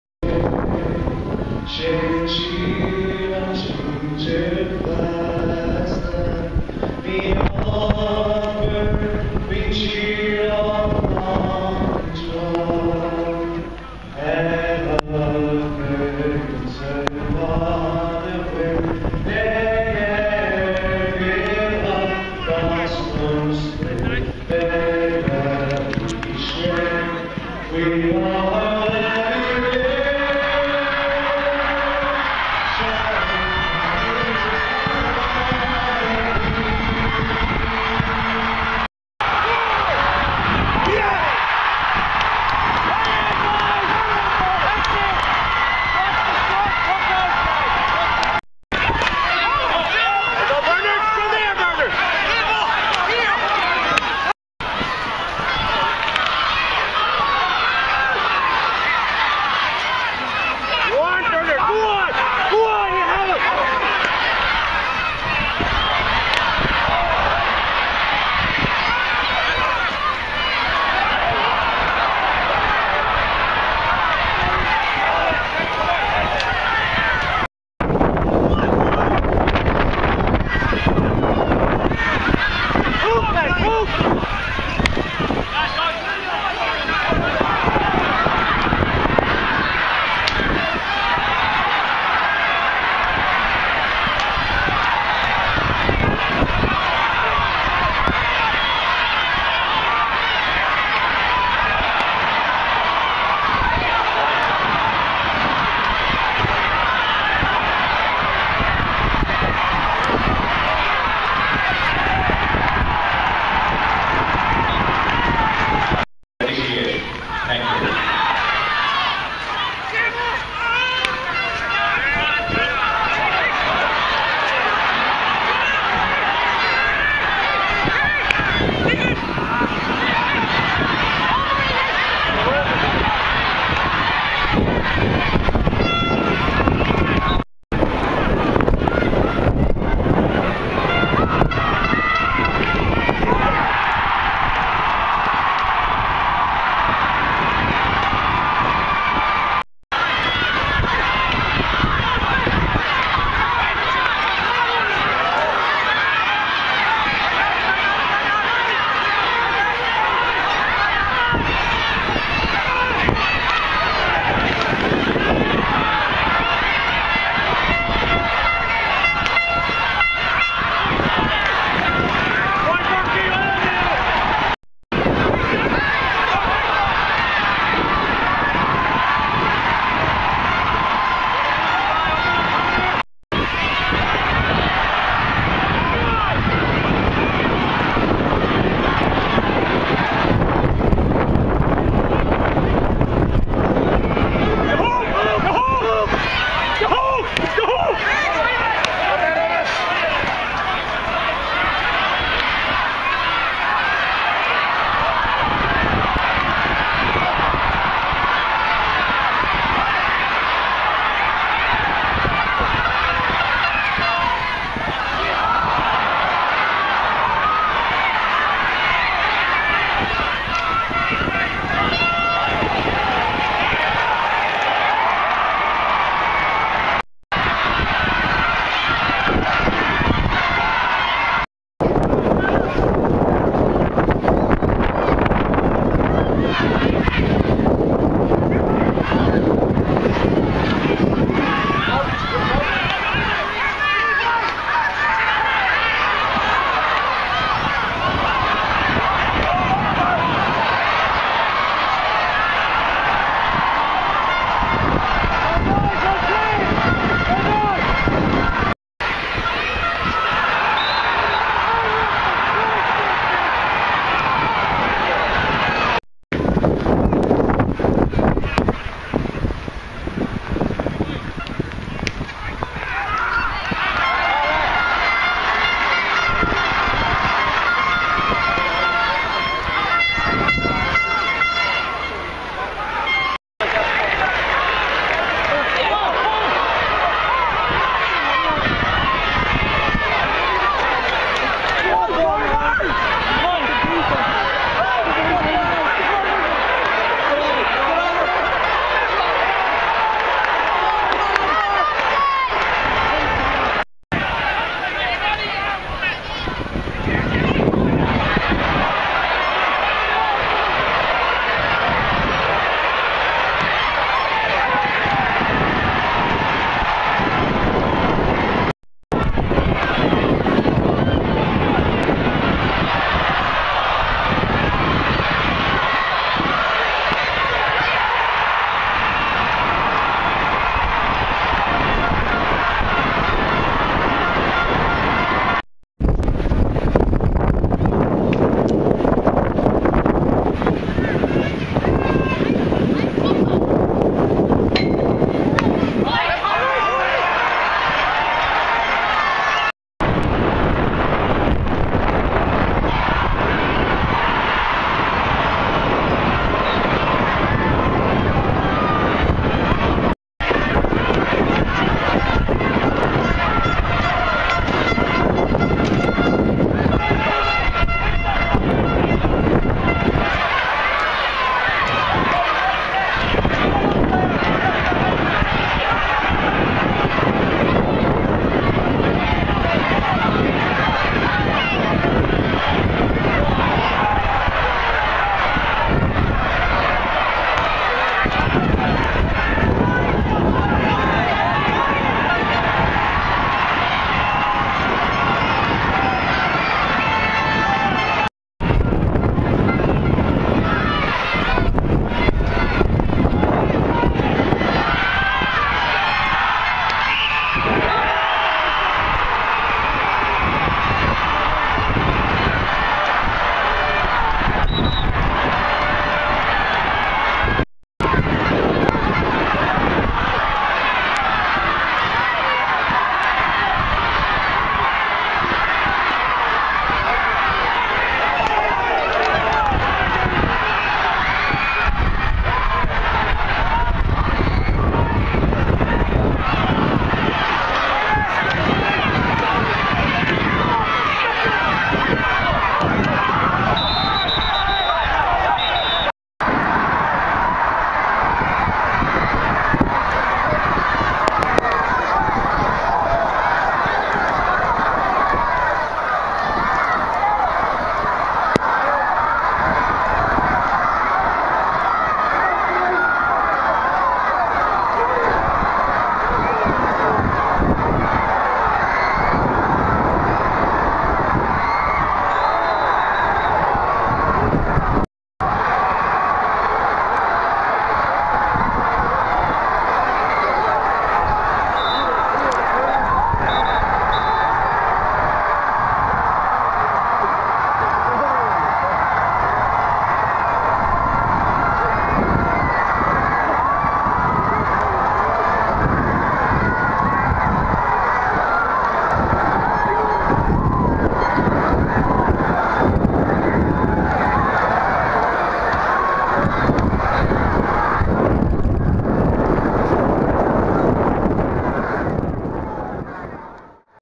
St Thomas's Galway Senior Hurling Champions 2012
St Thomas's win their first Galway Senior Hurling County Title.